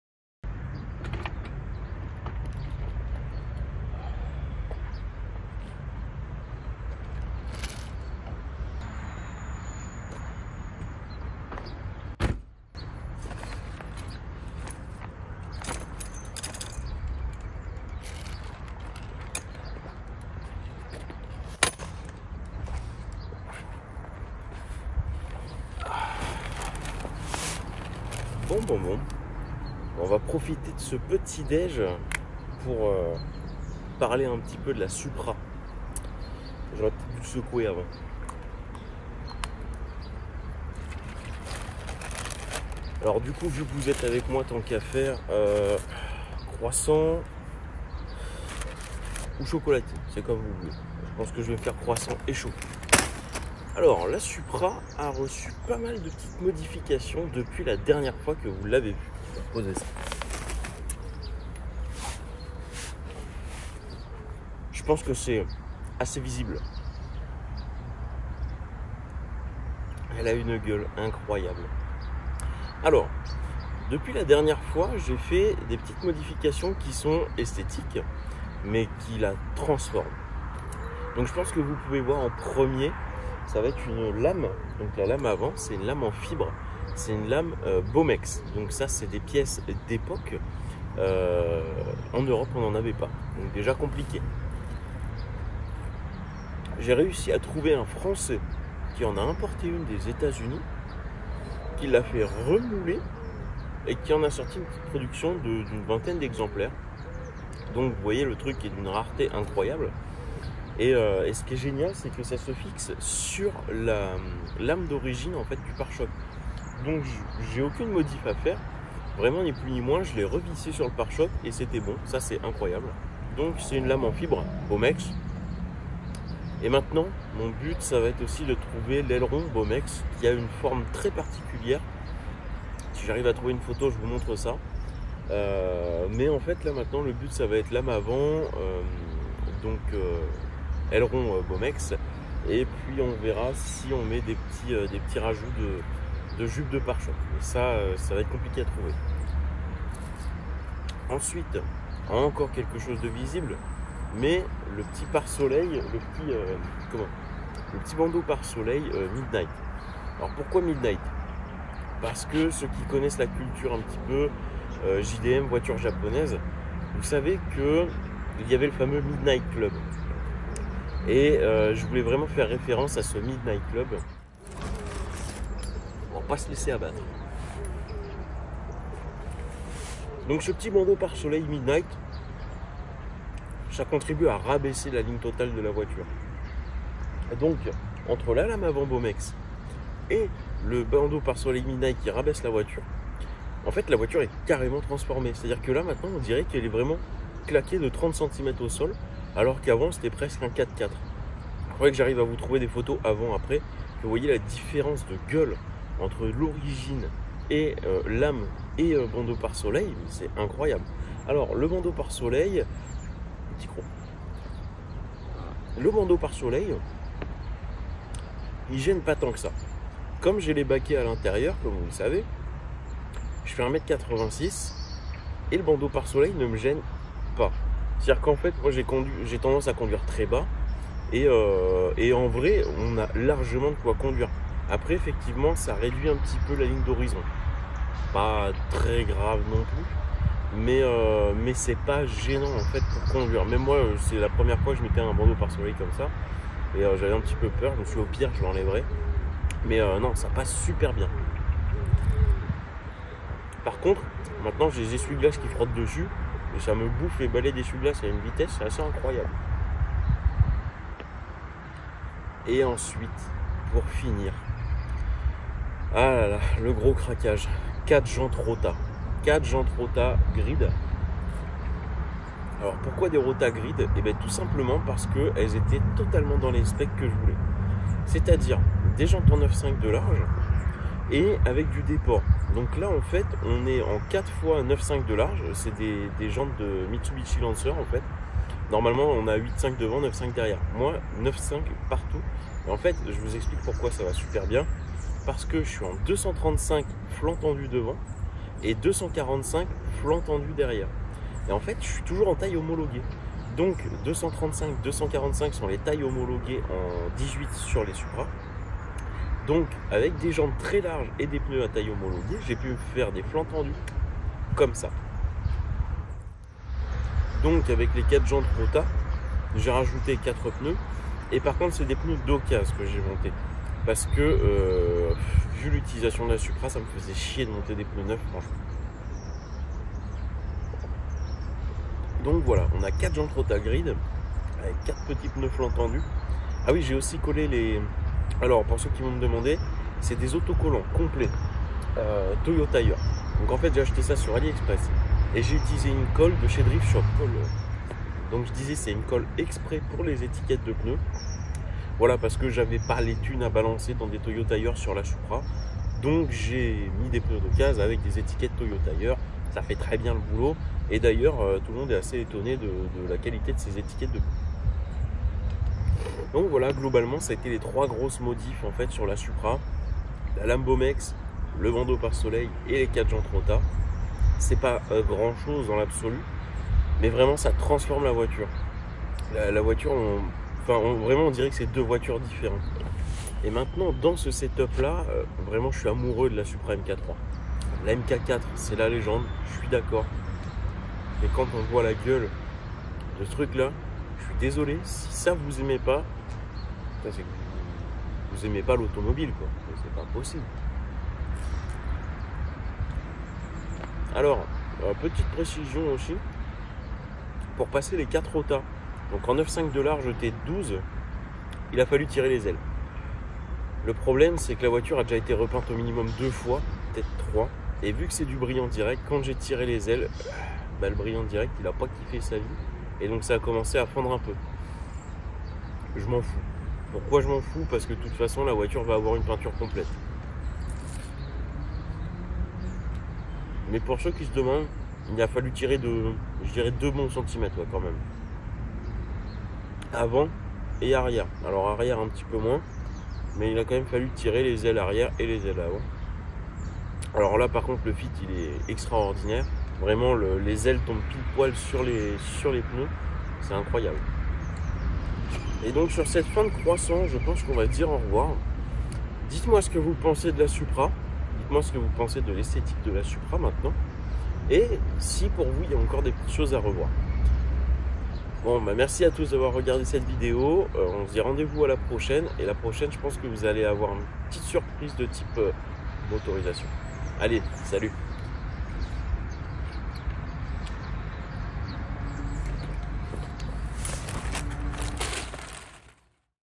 MA TOYOTA SUPRA DEVIENT FOLLE !
Bonjour a tous , on va se poser aujourd'hui pour parler un peu de l'esthétique de la supra . Ptit vlog posey , ptit dej tranquile !

➜ÉQUIPEMENT VIDÉO:
CAMÉRAS :